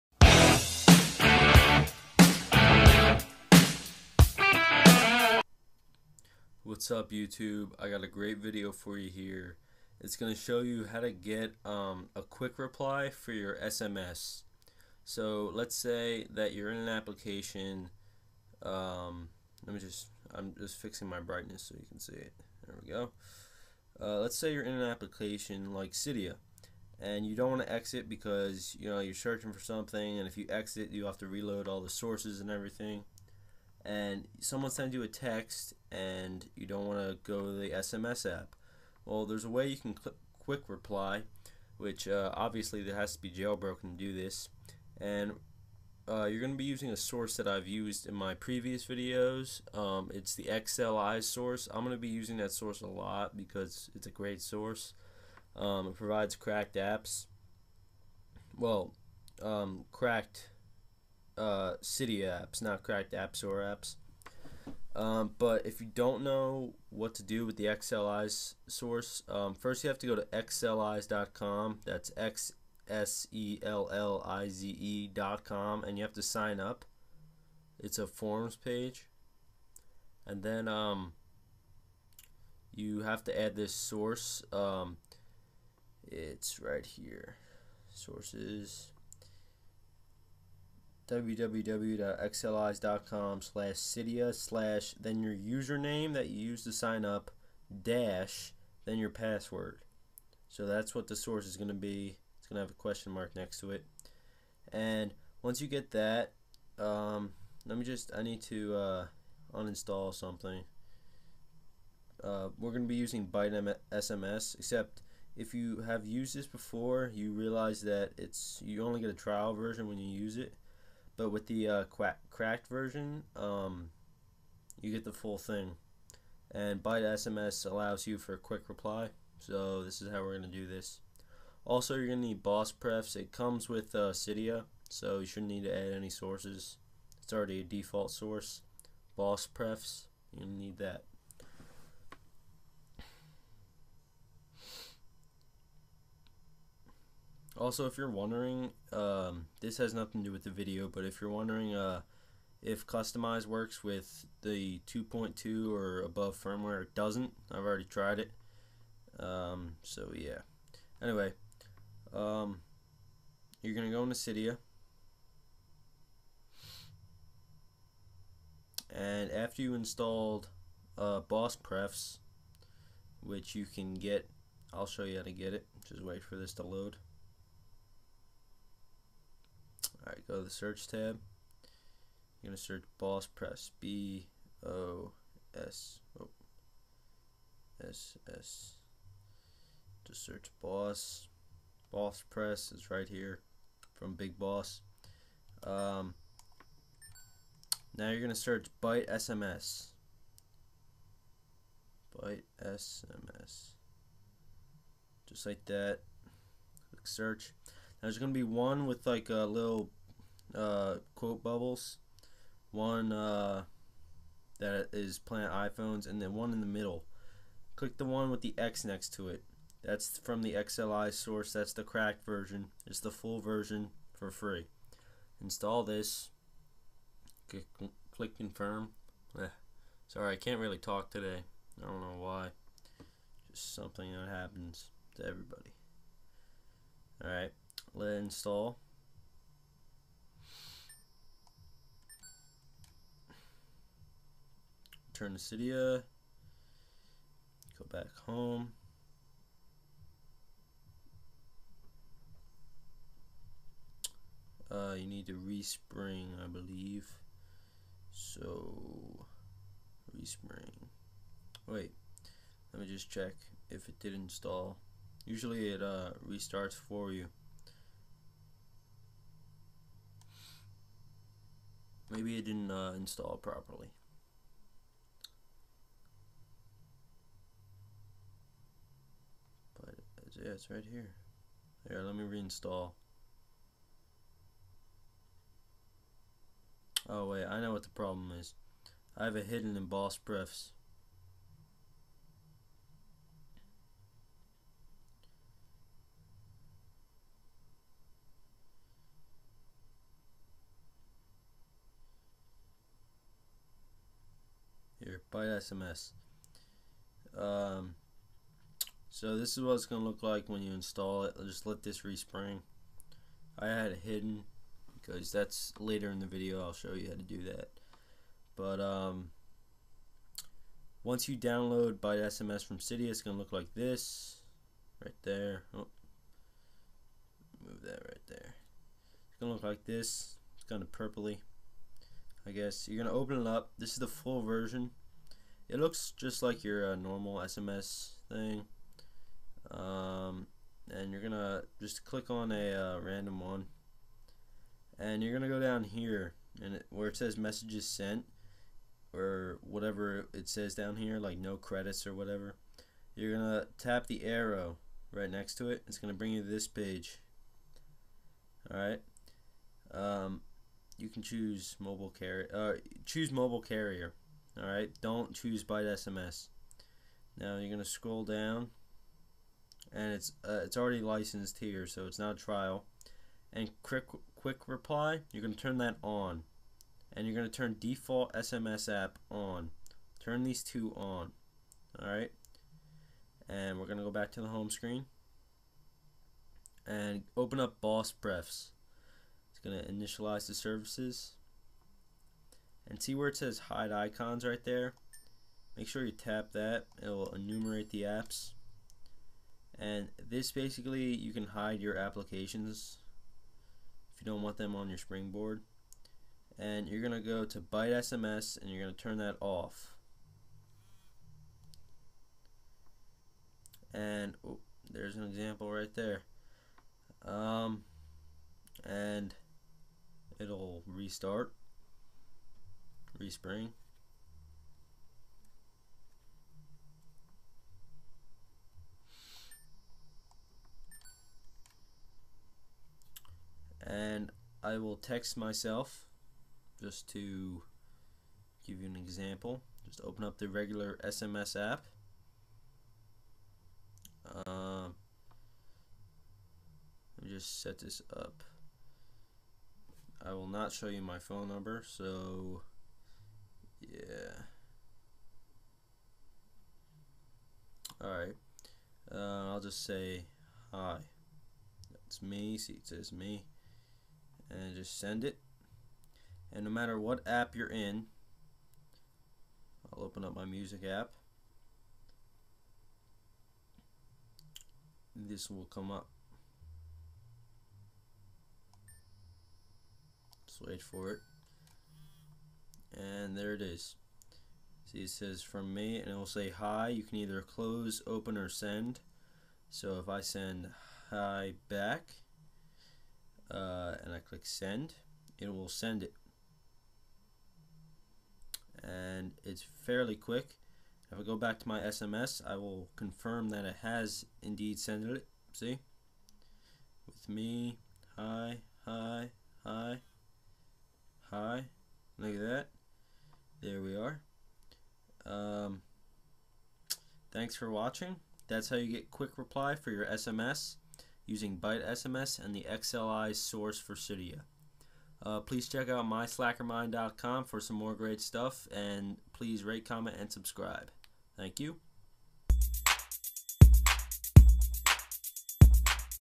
How to get Quick Reply for your SMS app
for some more great stuff.

THINGS YOU NEED:
BossPrefs
BiteSMS

replace USERNAME with your username and PASSWORD with your password